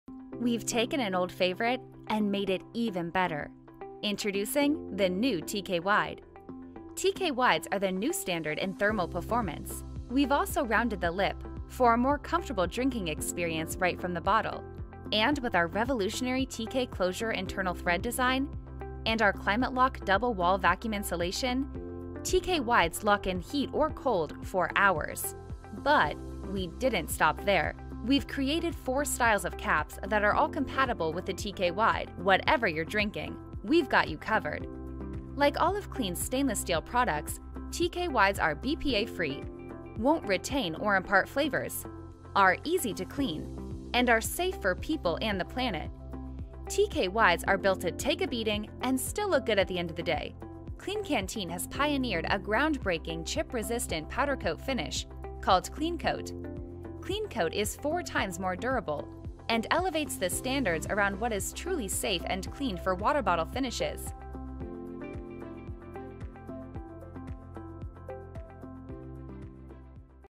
Botella térmica acero inoxidable TKWide - Klean Kanteen
Botella térmica de doble pared de acero inoxidable, con tapón TK Closure™, de Klean Kanteen. Para mantener los líquidos fríos o calientes durante más horas que en otros termos y poder tomarlos en cualquier parte ya que se puede beber directamente de la botella. Termo 100% libre de plásticos, de materiales seguros, diseño elegante y práctico.